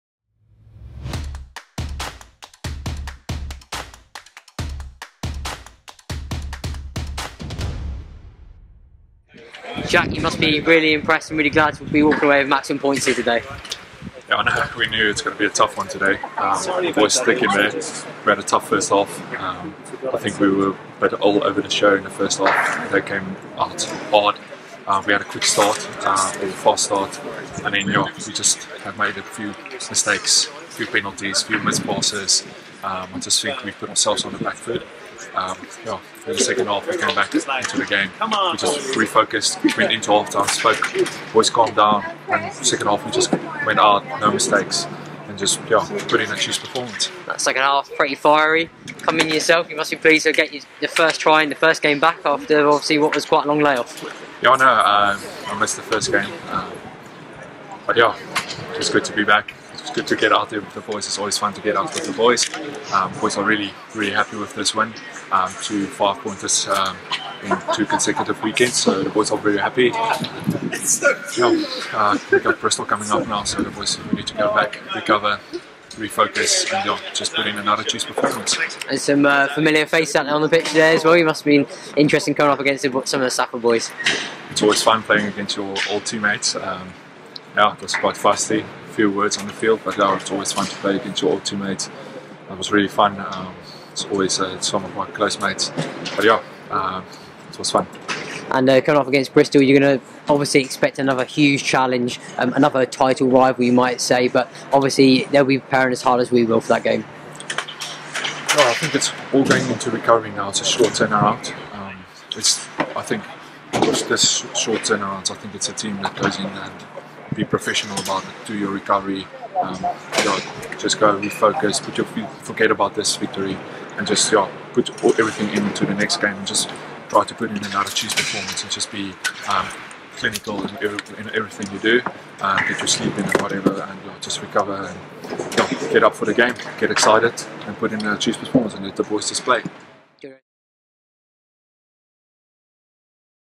Jacques Vermeulen post Sale Sharks
Chiefs TV - We catch up with Exeter Chiefs Jacques Vermeulen moments after the final whistle of our 22-32 victory over Sale Sharks this evening